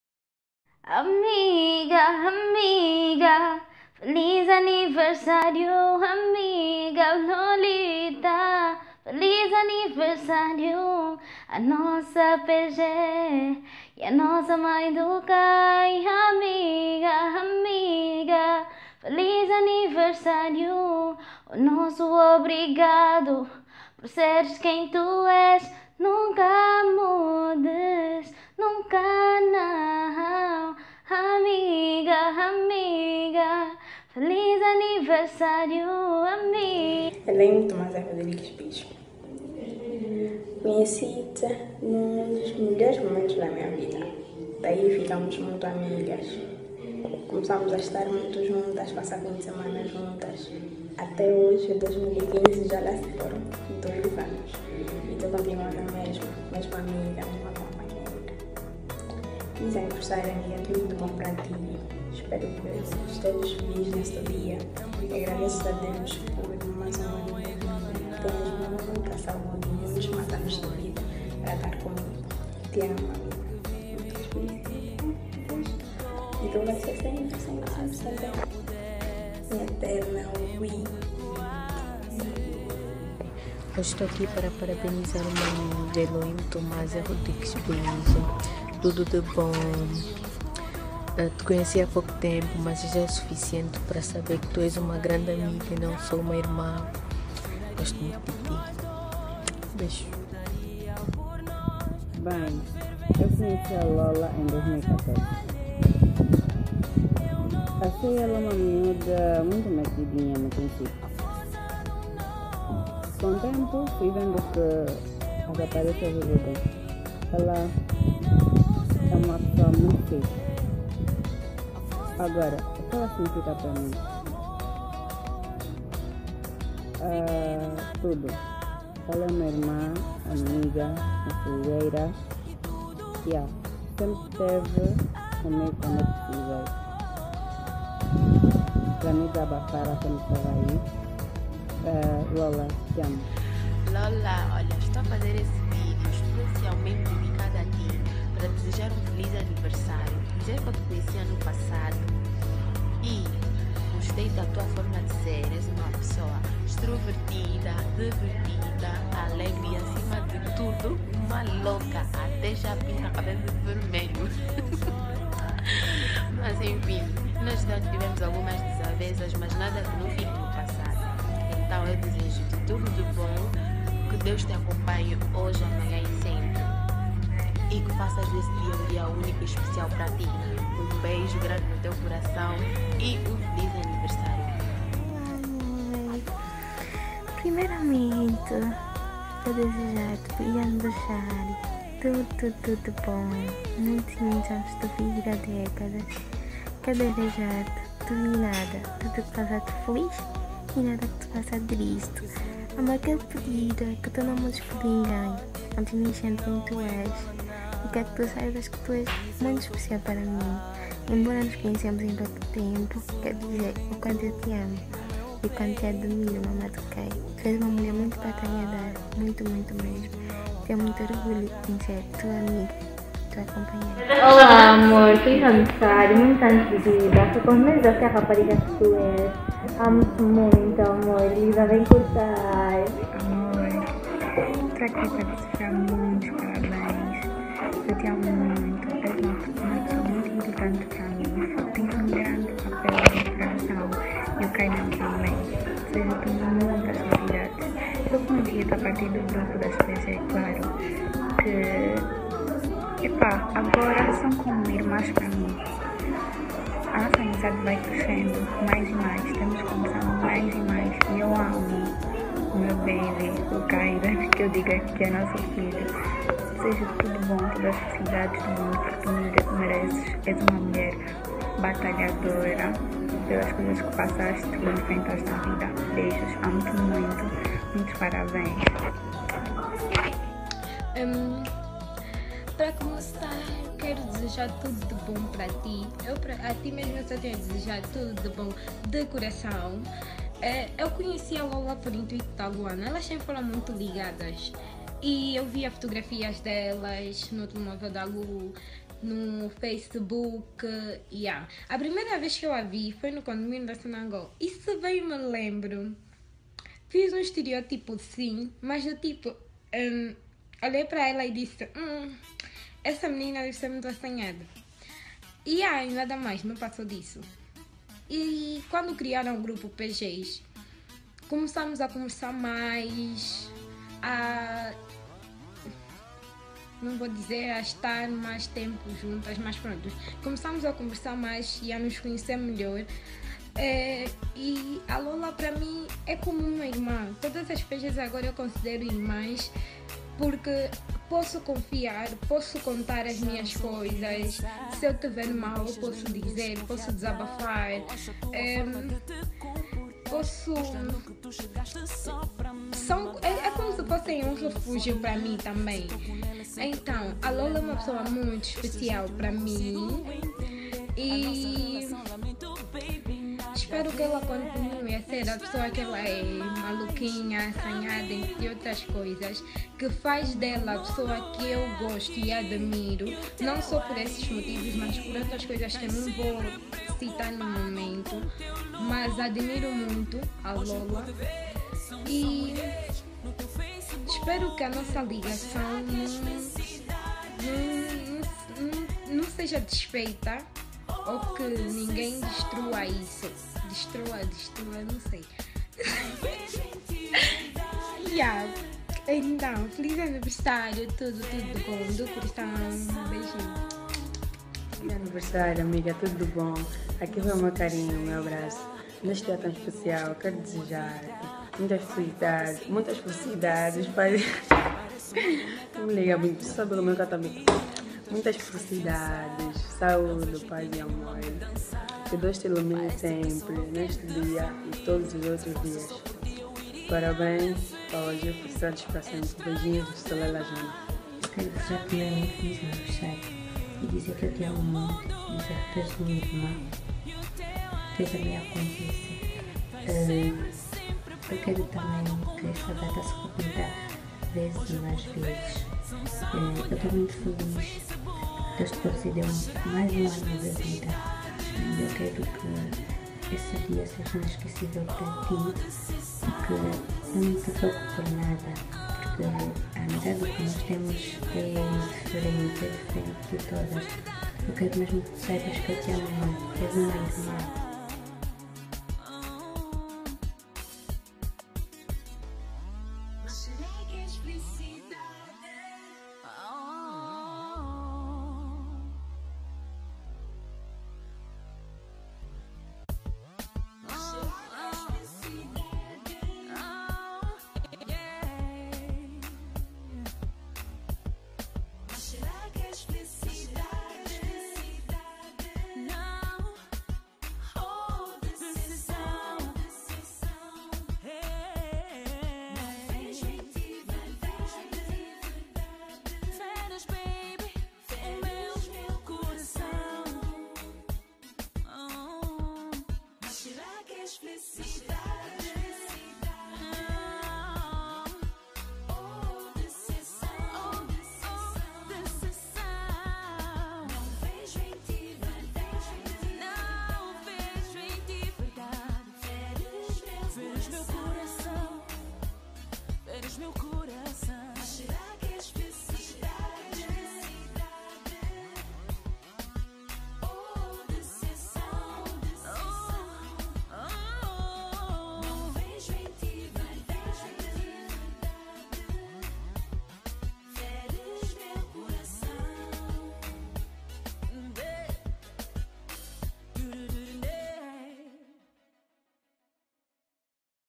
Feliz aniversario LOLA
Video feito pelas Pg pra desejar feliz aniversario pra ndeng Lola